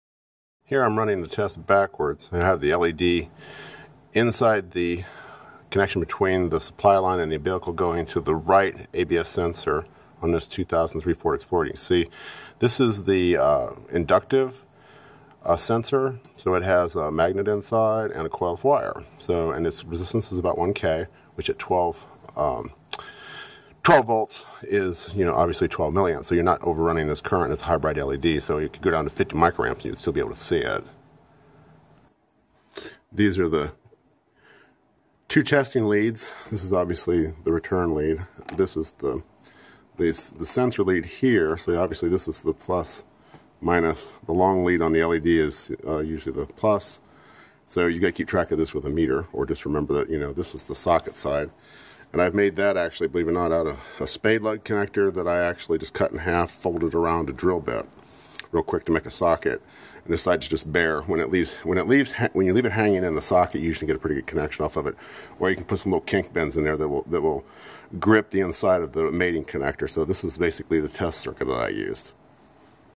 Simple General LED Test Circuit for ABS
Shows you the presence of voltage, continuity and signal with the change in amplitude magnet for an adductive coil ABS sensor. I'm using this on a 2003 Ford Explorer with a inductive sensing coil with a 1K ohm resistance. That puts the LED current at 12 milliamps which is nominal. You can use the same circuit on hall effect ABS sensors, you just need to make sure of the current is not excessive (for most high bright LEDs...less than 20ma). You also note that the variations on hall effect abs sensors or not the same. On both kinds of sensors you will get an indication as to whether the circuit is responding to the element and the metal teeth. This does not speak to the absolute signal strength which may be an issue if there is a problem.